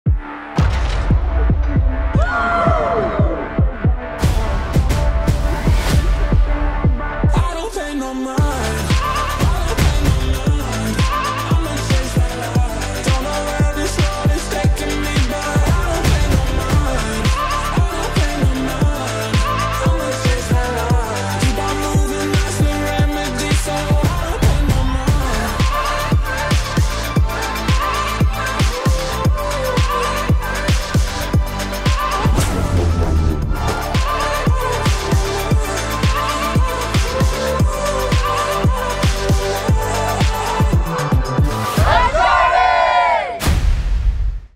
"Uncharted" Interactive Adventure with Sony Pictures - Nicque Marina
I still can't believe I got to do this! We were supposed to skydive for real but the weather was bad (OHHHHHHH noooooooo 😂😂😮‍💨)